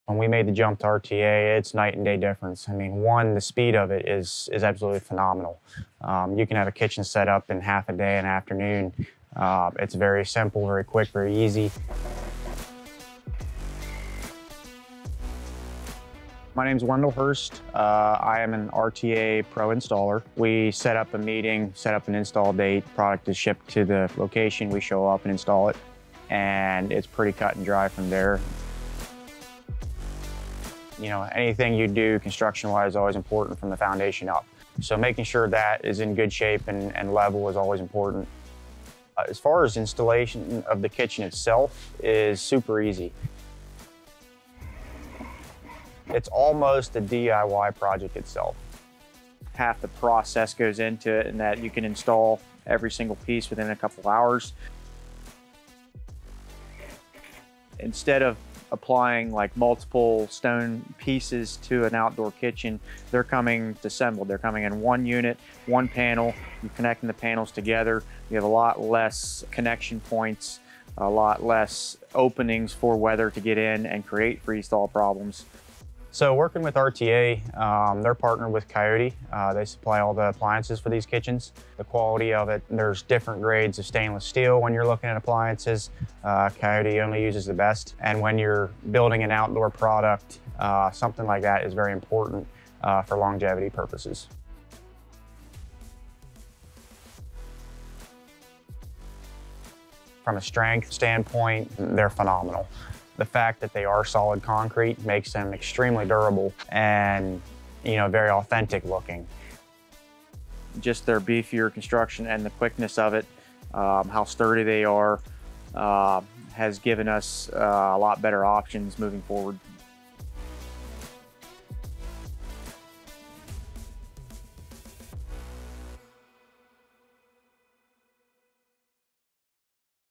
RTA Outdoor Kitchen Assembly | Demonstrated by a Pro Installer
When it comes to the RTA outdoor kitchen assembly… You might be feeling a bit unsure about the process. If you’re a homeowner, you might not be sure if you can really tackle this task yourself. If you’re a contractor… You might be assessing whether it’s worth becoming an RTA Pro Installer. No matter which camp you’re in, this video is for you. Pro Installer Wendell Hurst explains just how easy it is to assemble an RTA outdoor kitchen, whether you’re a contractor… Or looking to hire one to take care of assembly.

The first thing you’ll hear Wendell mention about the RTA outdoor kitchen assembly is how fast it is. Traditionally, a scratch-built outdoor kitchen could take weeks or even months to build. Depending on when you get started, that means you could end up missing out on the entire barbecue season! But with RTA, it will only take about half a day to get your entire grill island assembled and ready to use. It’s obvious to see how significant of a time savings this is! Not only does this save time on the part of the contractor and homeowner… It also saves homeowners a boatload of money on labor costs.

The RTA outdoor kitchen assembly process also makes it uniquely durable in the outdoor environment. Rather than being constructed with multiple layers that each  add a point of failure… RTA islands arrive at your door as a single, solid panel. This means that moisture can’t penetrate into the panels, causing delamination and other issues. But that’s just the start! Watch the rest of the video for more details.